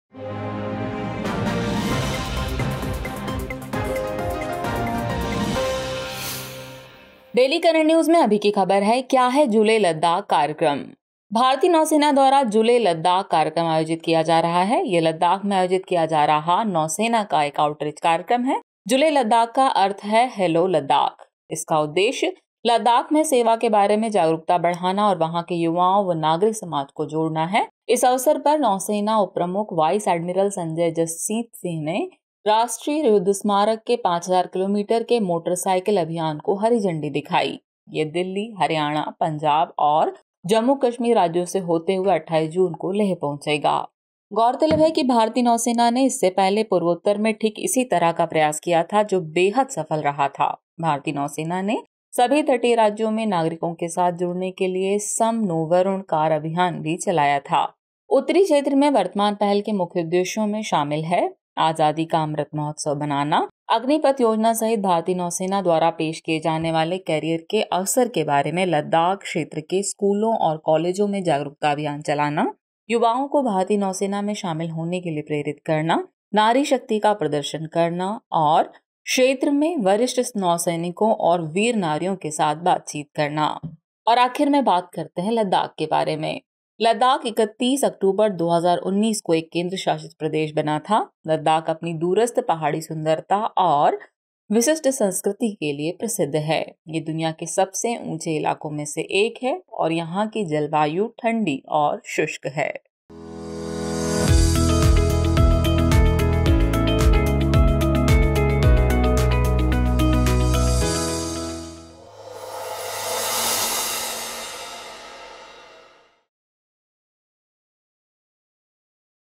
What Is ‘Julley Ladakh’ Program : Daily Current News | Drishti IAS
It is an outreach programme to Ladakh to increase awareness about the service in the pristine state and to engage with youth and civil society there.

अस्मिता मेन्स स्पेशल योजना में शामिल होने के लिये इस लिंक पर क्लिक करके रजिस्ट्रेशन करें :

1. Videos by Dr. Vikas Divyakriti

2. Concept Talk- By Dr. Vikas Divyakirti